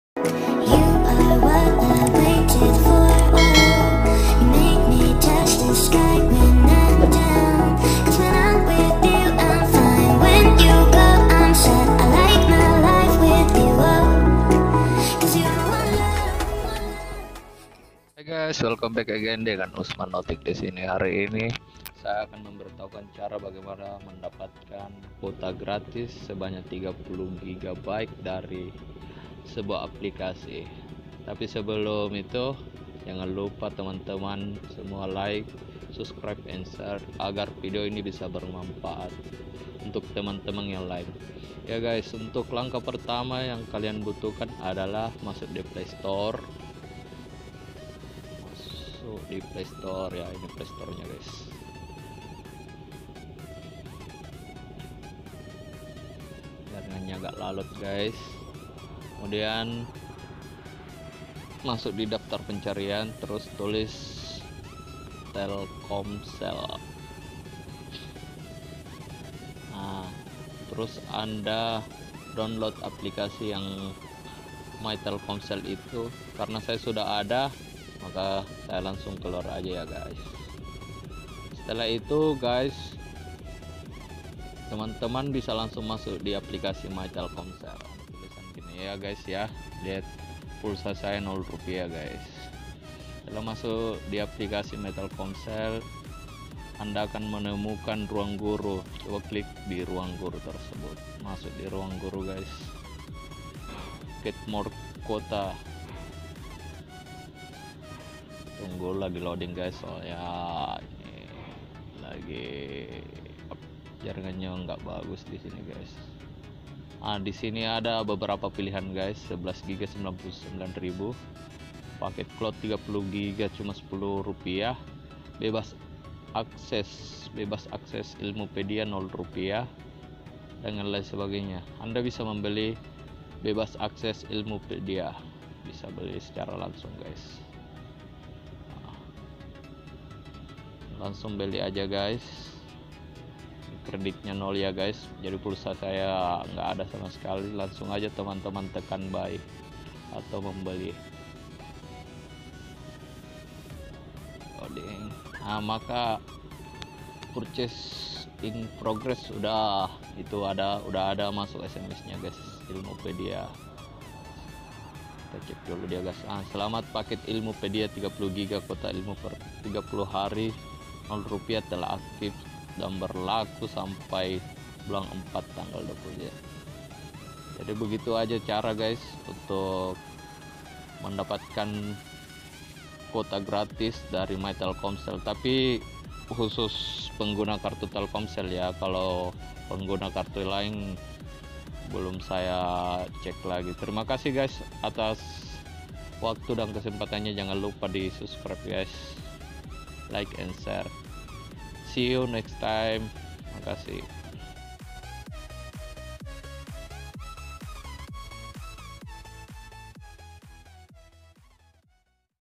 GRATIS!!! Cara mendapatkan kuota
Inilah tutorial cara mendapatkan kuota secara gratis...